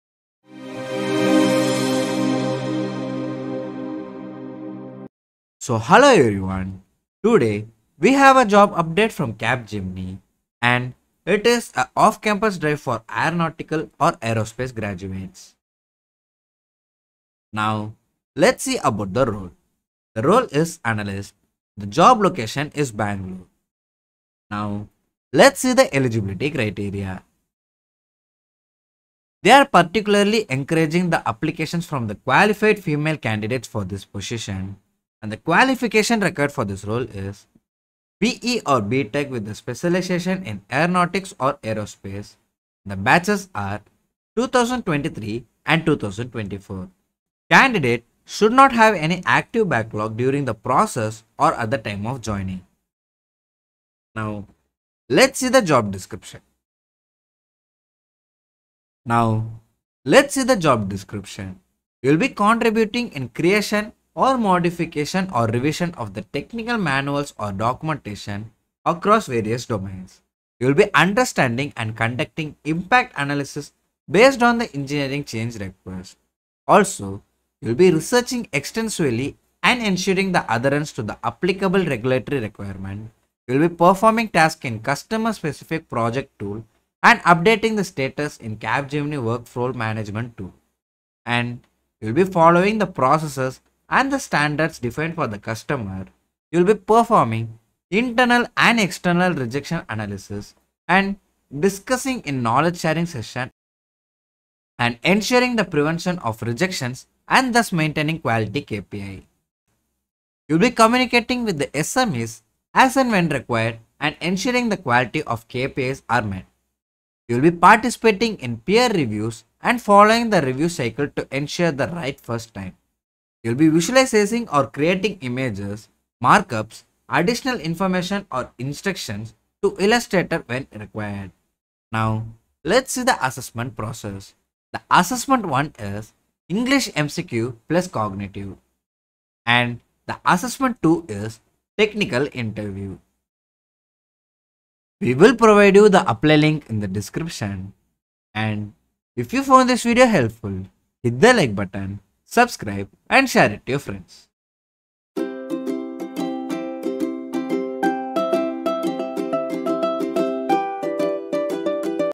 Capgemini Off-Campus Drive 2024 | Aeronautical & Aerospace Graduates | Job Role: Analyst | Apply Now
Looking for job opportunities as an Aeronautical or Aerospace graduate? 🚀 Capgemini is conducting an off-campus drive for 2023 and 2024 graduates with B.E./B.Tech degrees in Aeronautics or Aerospace. 🌟

In this video, learn everything you need to know about applying for the Analyst role at Capgemini, a global leader in technology transformation with 340,000 team members in over 50 countries. The job location for this exciting opportunity is in Bangalore, India.

Eligibility Criteria:

B.E./B.Tech in Aeronautics or Aerospace (2023 & 2024 Batch)
No active backlogs during the hiring process or at the time of joining
Applications from female candidates are particularly encouraged as part of Capgemini’s diversity and inclusion efforts
Job Responsibilities:

Create and modify technical manuals and documentation
Conduct impact analysis based on engineering change requests
Ensure adherence to regulatory requirements
Use project and workflow management tools to update status
Participate in peer reviews and maintain high-quality KPIs
Assessment Process:

Assessment 1: English MCQ + Cognitive Test
Assessment 2: Technical Interviews
🔒 Important: Candidates must complete the registration process, including Aadhar card verification and have an active DigiLocker account to apply.

This is a fantastic opportunity for Aeronautical and Aerospace graduates to join a leading global tech company and kickstart their careers! Watch the video to learn more and how to register for the off-campus drive. 🚀